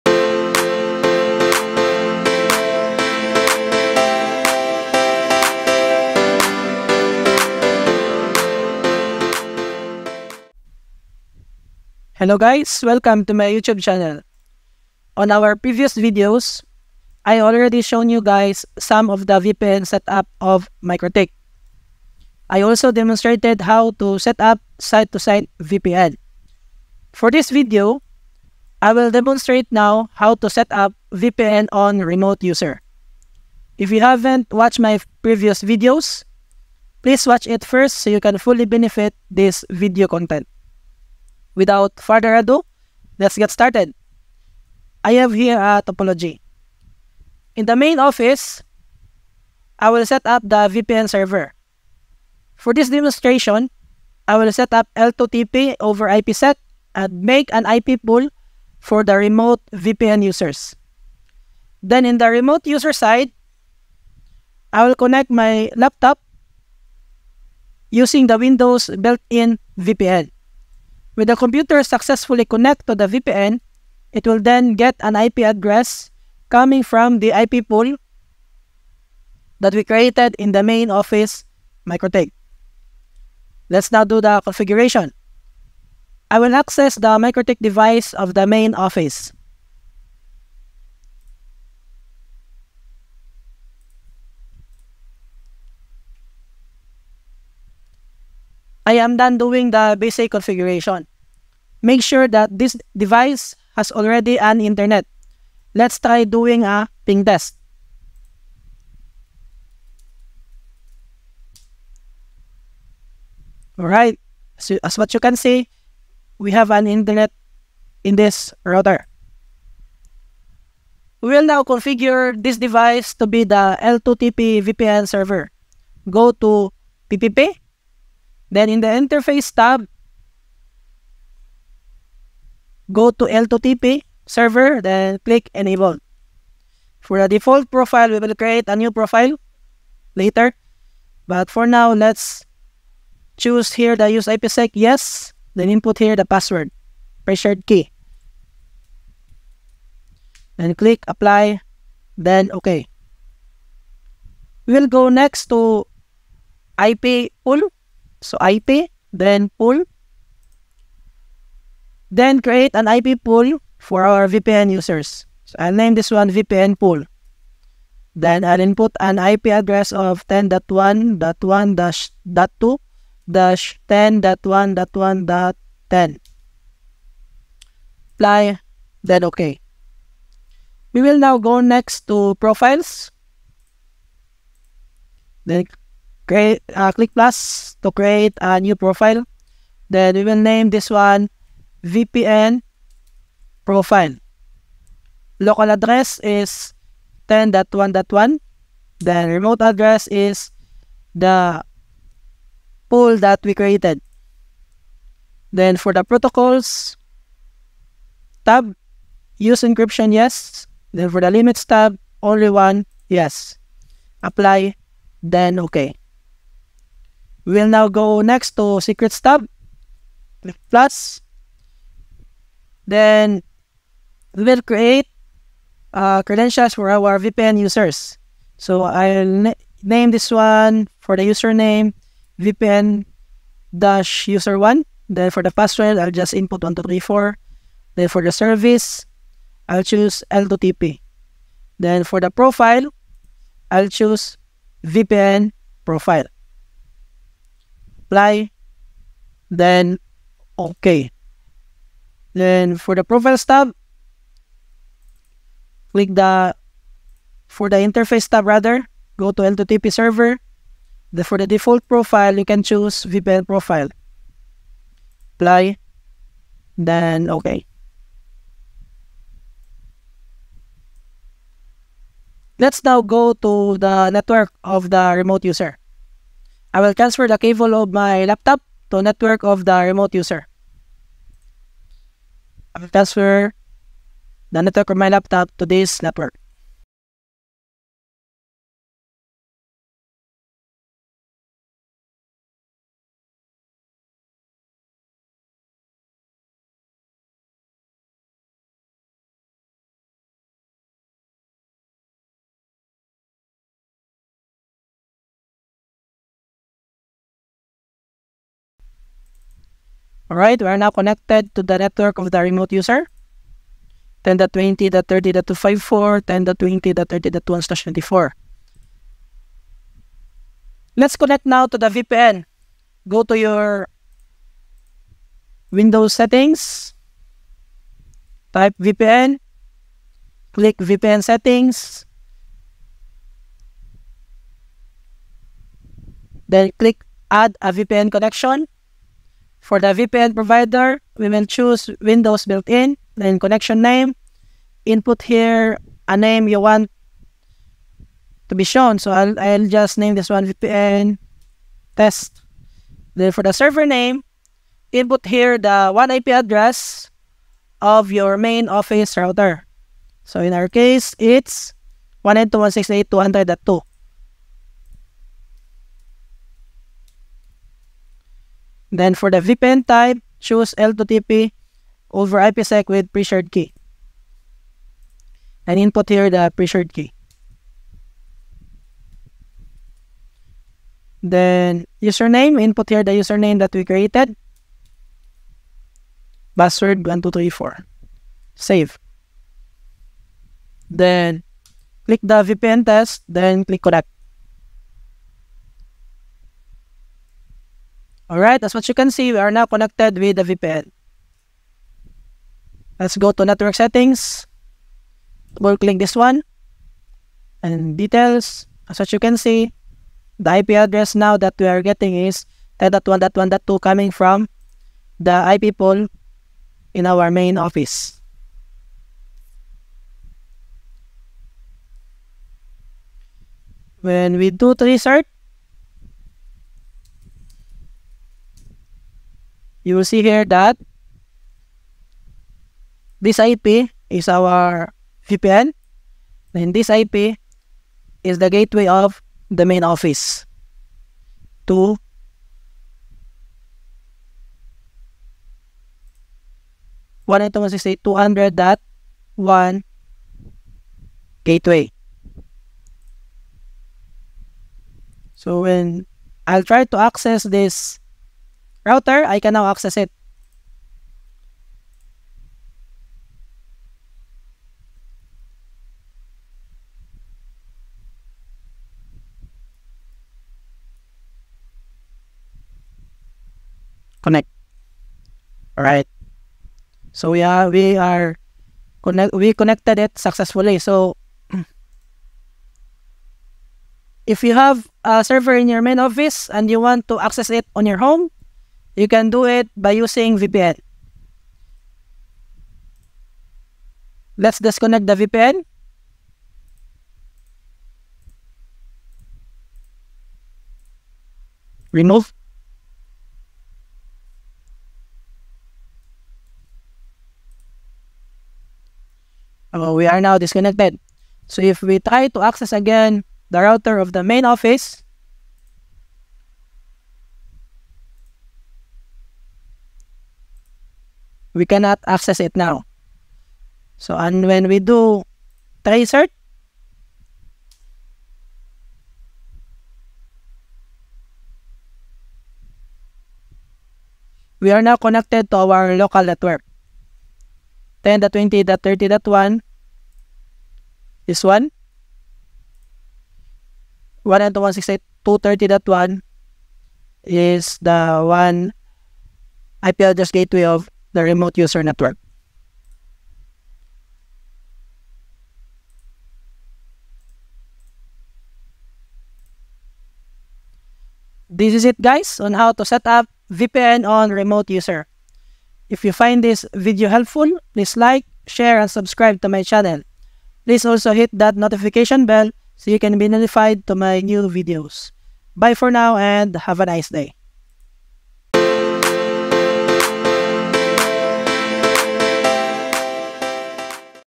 Mikrotik VPN - VPN on Remote User | Mikrotik Configuration Tutorial Step by Step
Learn how to setup VPN on Remote User in Mikrotik.

In this video tutorial, I will show you guys how to setup VPN on Remote User in Mikrotik.

If you feel I missed anything or that I can do a better job in some area, I will be happy to consider your recommendations.